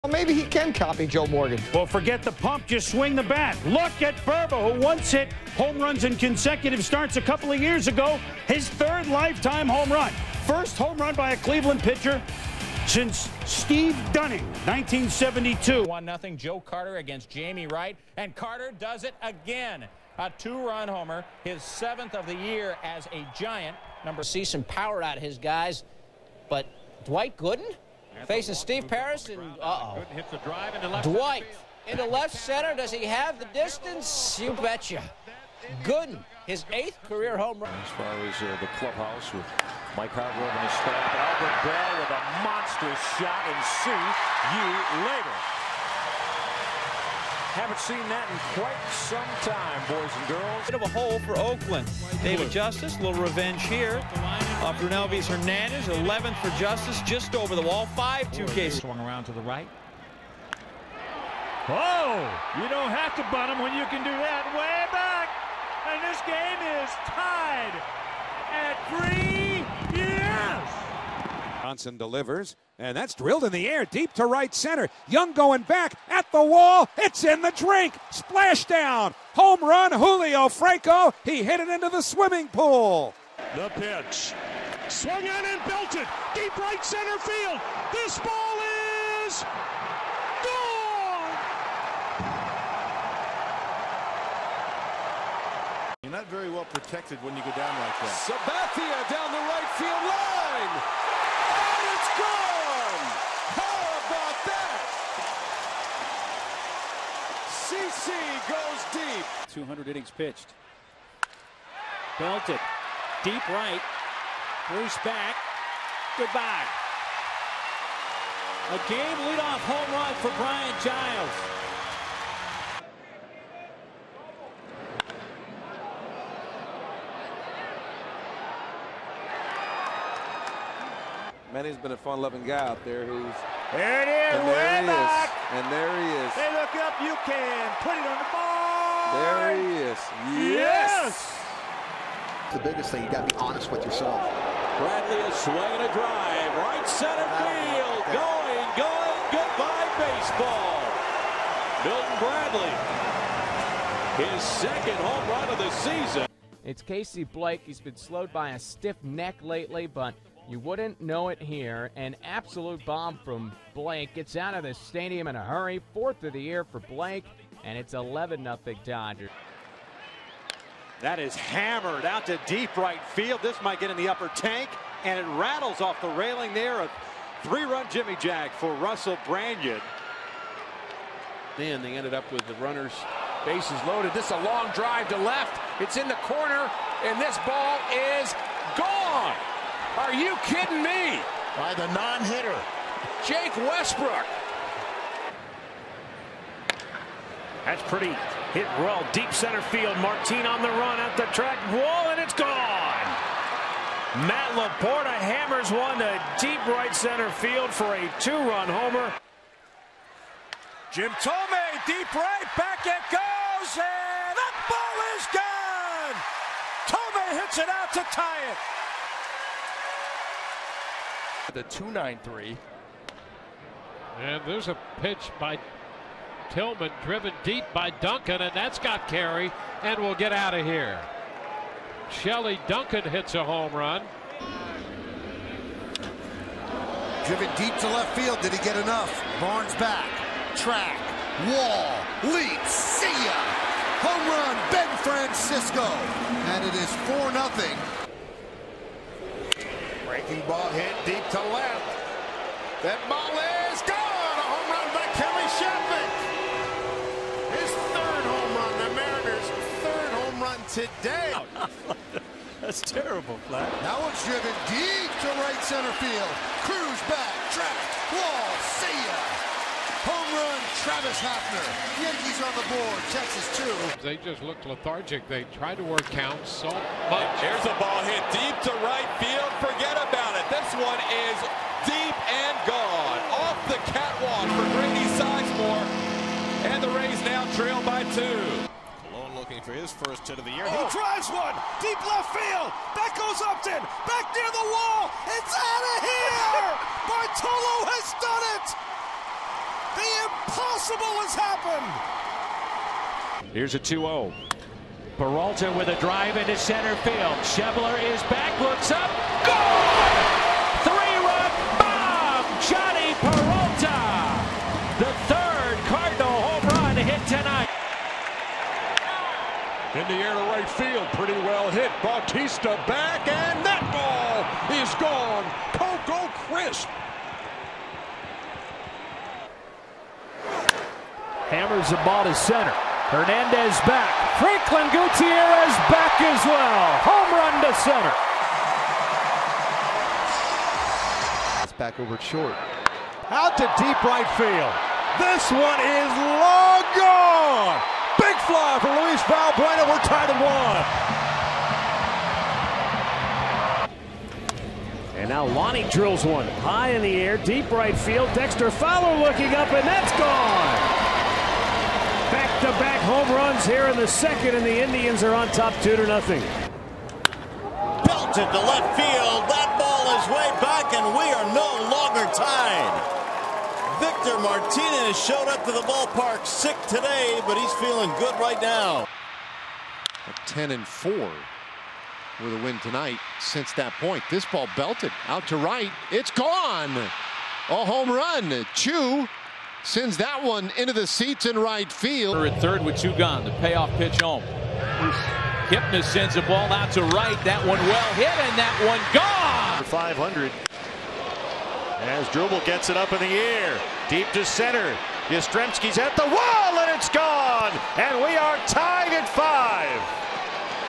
Former Indians' Final Major League Home Runs
Compilation of notable former Cleveland Indians players hitting their final Major League Home Runs:

Dave Burba (6/7/98)
Joe Carter (9/27/98)
Doc Gooden (6/11/99)
Albert Belle (10/1/00)
David Justice (9/2/02)
Roberto Alomar (8/7/04)
Julio Franco (5/4/07)
Kenny Lofton (2007 ALCS Gm 3)
CC Sabathia (7/13/08)
Brian Giles (5/16/09)
Omar Vizquel (9/8/10)
Manny Ramirez (9/17/10)
Milton Bradley (4/18/11)
Casey Blake (6/27/11)
Russell Branyan (8/26/11)
Jake Westbrook (8/31/11)
Matt LaPorta (9/13/12)
Jim Thome (9/26/12)
Shelley Duncan (4/17/13)
Ben Francisco (5/1/13)
Kelly Shoppach (5/27/13)
Travis Hafner (6/25/13)
Grady Sizemore (8/12/15)
Bartolo Colon (5/8/16)
Jhonny Peralta (9/27/16)
Coco Crisp (2016 ALCS Gm 5)
Franklin Gutierrez (5/2/17)
Luis Valbuena (6/23/18)
Lonnie Chisenhall (6/27/18)
Victor Martinez (9/3/18)
Shin Soo Choo (9/5/20)
Jason Kipnis (9/6/20)
Asdrubal Cabrera (8/10/21)

Some clips courtesy of SW561